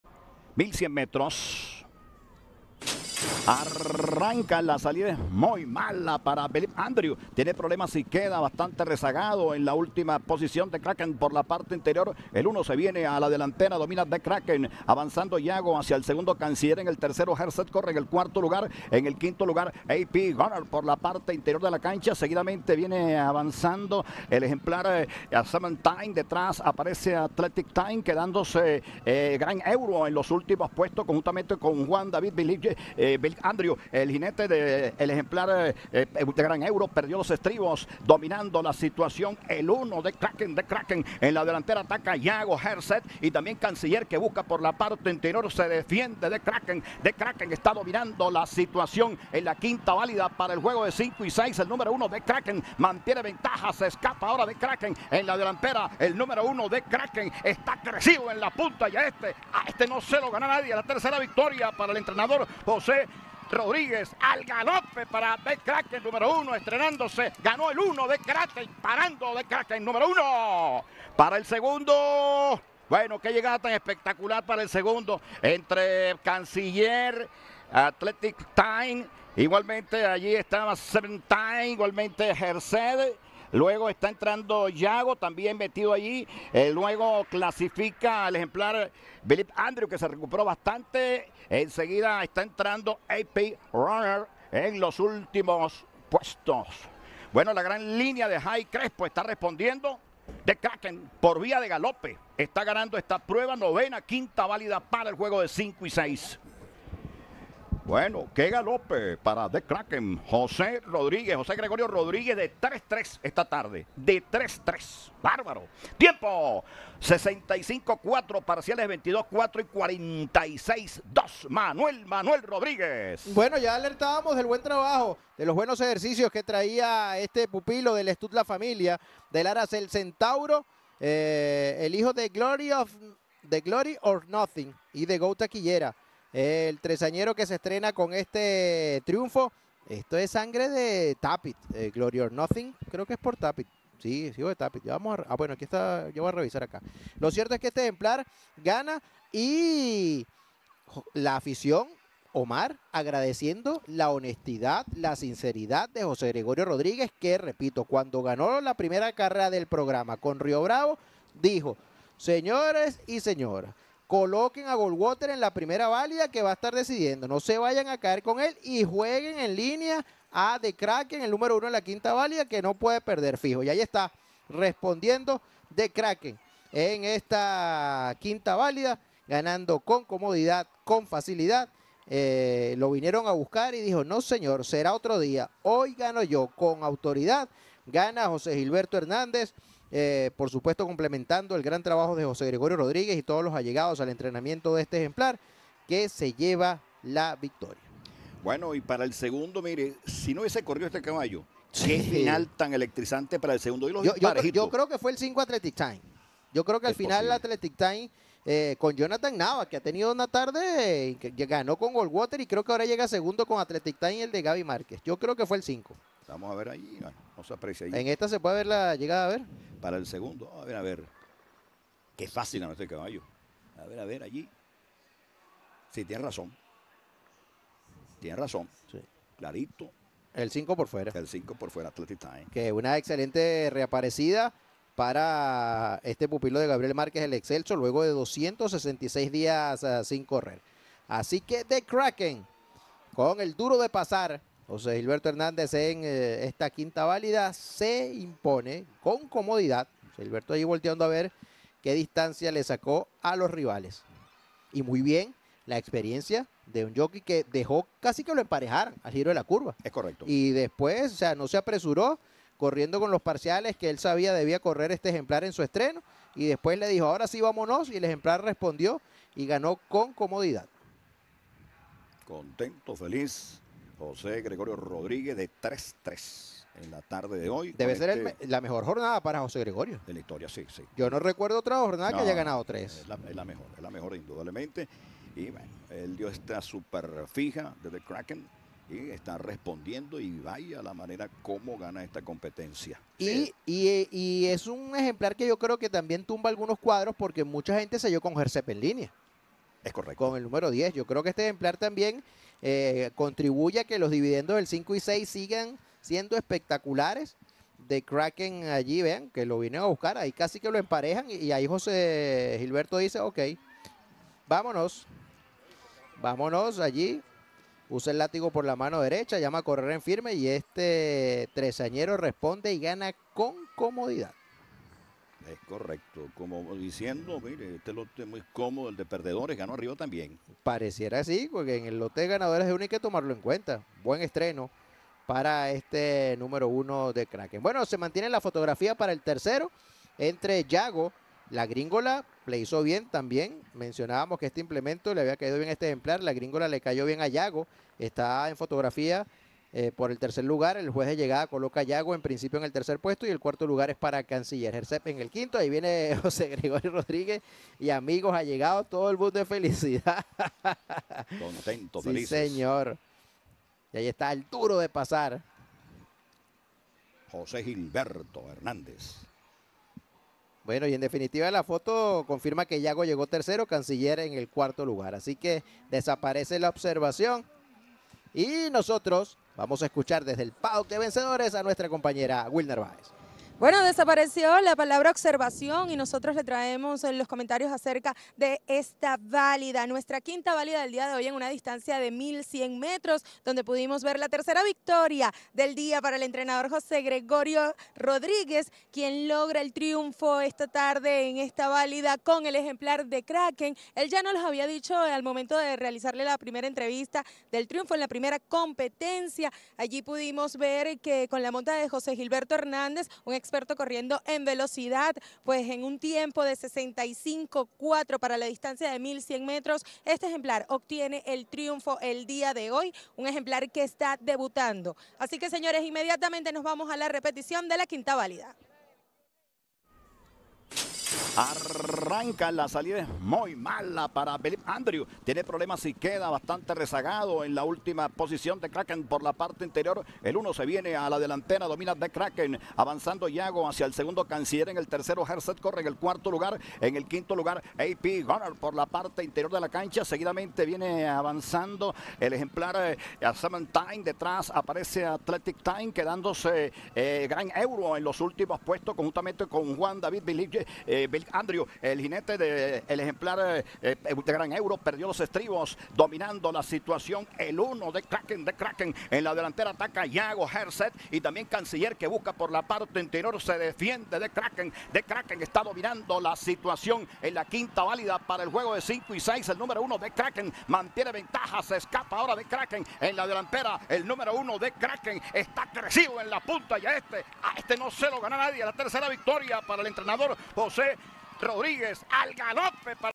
9na. carrera | 5ta. Valida | The Kraken | Hipódromo La Rinconada | Reunión 30 | 2024
1: The Kraken (1) | Jinete: J.G. Hernandez | Dividendo: 54,97
2: Athletic Time (5) | Jinete: J. Nava
3: Yago (4) | Jinete: E. Guedez
4: Canciller (3) | Jinete: H. Medina
5: Gerseb (10) | Jinete: R. Capriles
Distancia: 1100